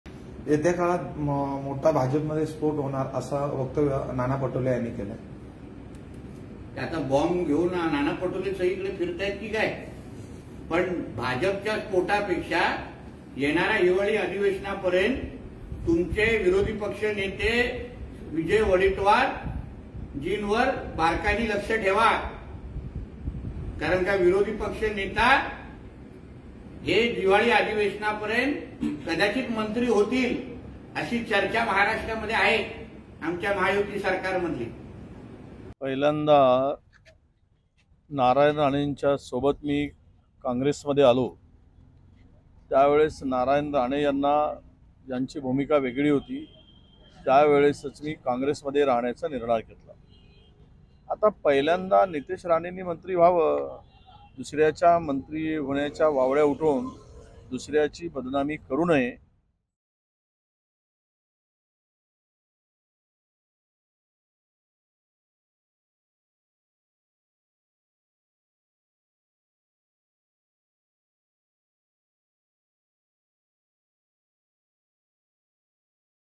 Nitesh Rane vs Vijay Wadettiwar : भाजप प्रवेश अन् मंत्रीपदावरून तू तू, मैं मैं | LetsUpp Marathi
विरोधी पक्षनेते विजय वडेट्टीवार भाजप प्रवेश करतील असा दावा भाजप आमदार नितेश राणे यांनी केला होता. त्यावर वडेट्टीवारांनी त्यांना प्रत्युत्तर दिले आहे.